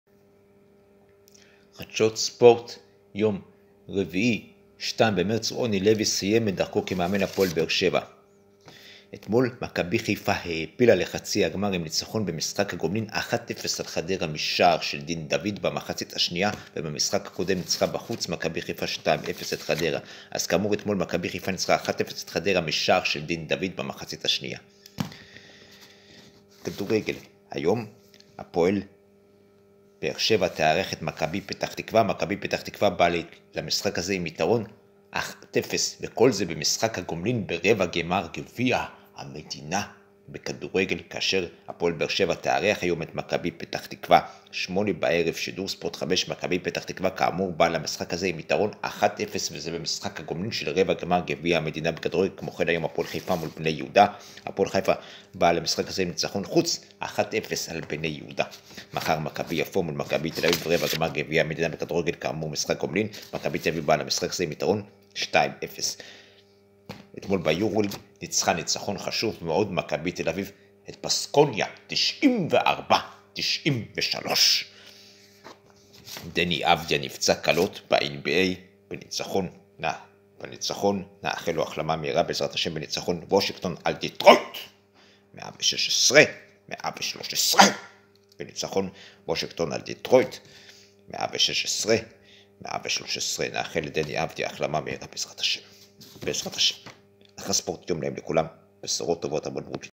יום 2517 -חדשות ספורט רביעי 02.03.22 -
ב"ש  2  מכבי  פ"ת   0   ב"ש  עלתה  לחצי  הגמר
הפועל  חיפה  1   בני  יהודה  0  הפועל  חיפה  עלתה  לחצי  הגמר